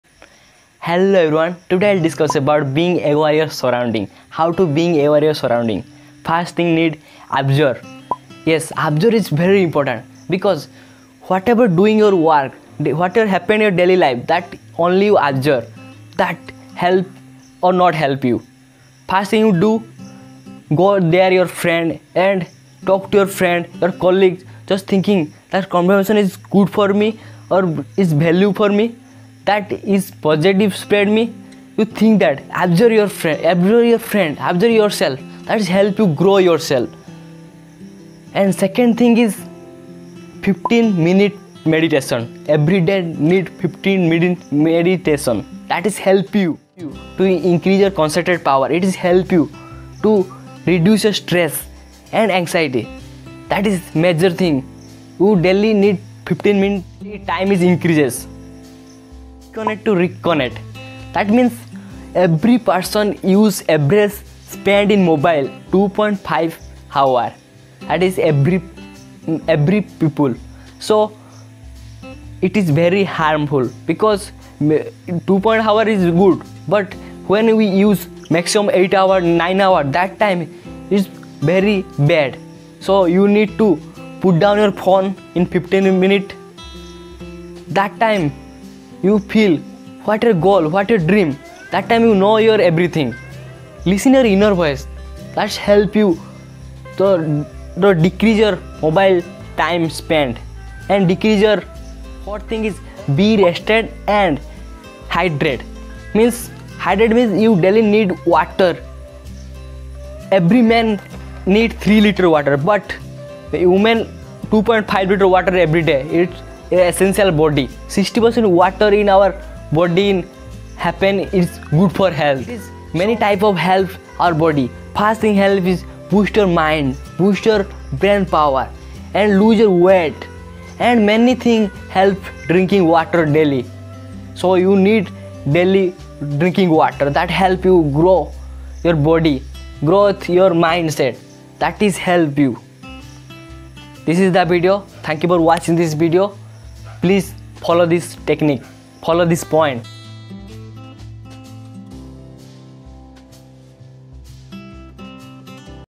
Be Aware For Your Surroundings
Topic- Be Aware Of Your Surroundings

Following this point -"be aware of your surrounding "

1)ABSORB

2)DISCONNECT TO RECONNECT -HARMFUL OF CELL PHONE

3)MEDIATE FOR 15 MINTUTES A DAY

it will help reduce your... -

ANXIETY

,INCRESSES CONCENTRATION

 LOWER RISK OF HEAL TH ISSUE

AND ALLOW YOURSELF MORE AWARE IN YOUR SURROUNDING

BE WELL RESTED HYDRATED-

1) POOR NUTRIATION

2), INJURES, LACK OF CONCENTRATED,

3)WHEN TIRED DOING SOME MISTAKES .

 WHY IS DRINKING WATER IMPORTANT?

REGULAR DRINKING WATER HELPS

-lose weight,

think better,stay in better mood,

 prevent disease and more

WHY OUR BODIES NEED WATER?-helps maintains blood volume,regulate our body temp,

water six benefits

 1)improve our physical performance help you lose weight

2 )boost your mood

3)boost your brainpower

4 )prevent headache

5)protect against disease-likes KIDNEY STONE, CONSTIPATION, ASTHMA, URINARY TRACT INF ACTIN, MAIE3 LITER AND FEMALE-2.21 lite

 4)TIMING IS EVERYTHING AND TRUST YOUR GUT (INNER Voice

[give more information on video topic